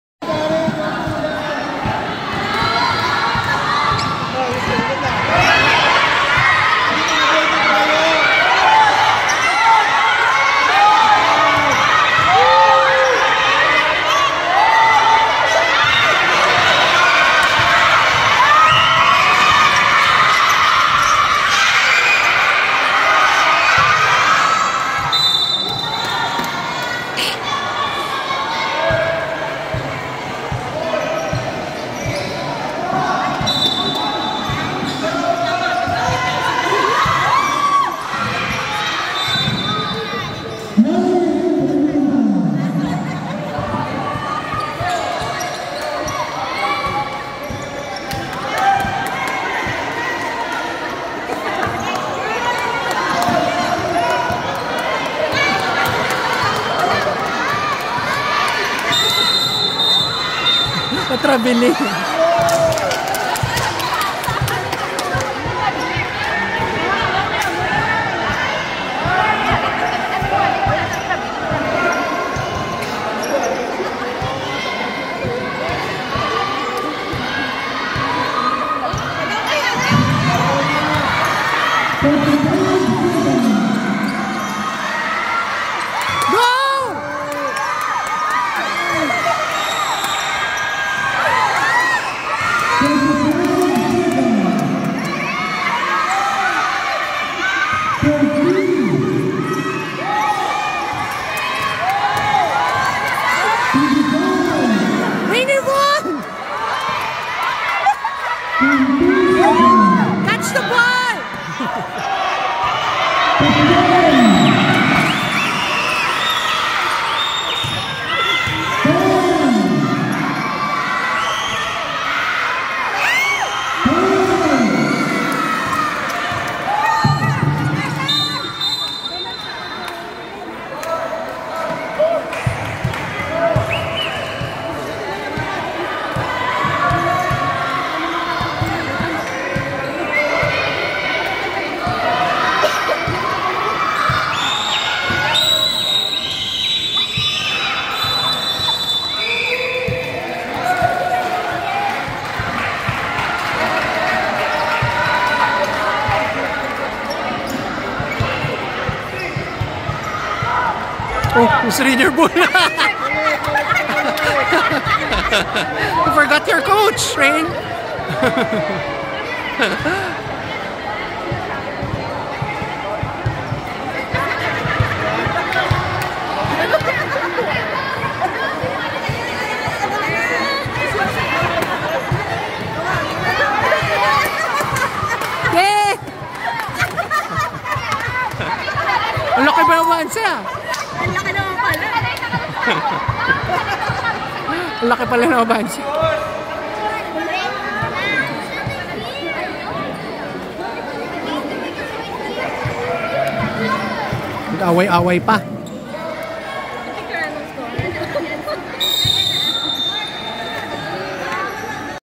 NOPSSCEA 10-Under Basketball Game 2023 - LCC-Bacolod VS. St. John's Institute(4)
Negros Occidental Private Schools Sports Cultural Educational Association